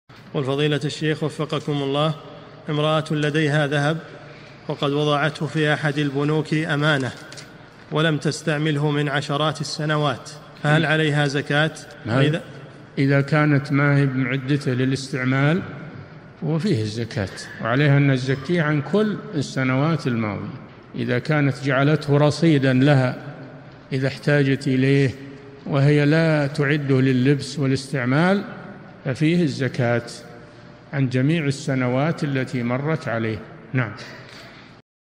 [854 -1024] امرأة وضعت ذهبها في أحد البنوك أمانة، ولم تستعمله من عشرات السنوات، فهل عليها زكاة؟
-[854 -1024] السؤال: امرأة لديها ذهب، وقد وضعته في أحد البنوك أمانة، ولم تستعمله من عشرات السنوات، فهل عليها زكاة؟
الجواب: إذا كانت ما هي معدته للاستعمال فهو فيه الزكاة وعليها أن تزكي عن كل السنوات الماضية إذا كانت جعلته رصيدًا لها إذا احتاجت إليه وهي لا تعده للبس والاستعمال ففيه الزكاة عن جميع السنوات التي مرت عليه.
للاشتراك في قناة الشيخ صالح الفوزان
سؤال وجواب من شرح الفتوى الحموية ( 1024 ) مقطع

للاستفسار والإقتراحات والدعم
رئيس الفريق
لمشروع كبار العلماء